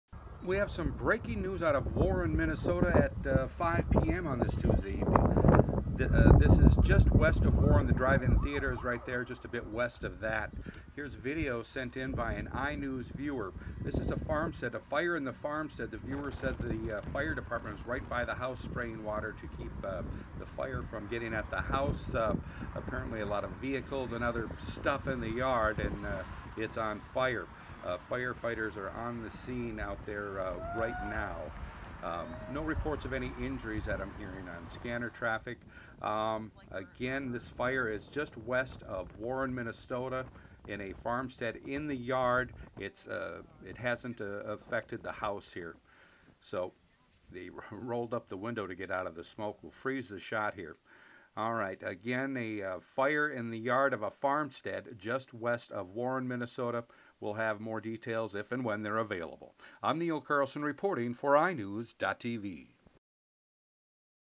BREAKING NEWS: Fire At Farmstead Near Warren, Minnesota
WARREN, MN (iNewZ.TV) Crews battling a large yard fire at a farmstead near Warren, Minnesota.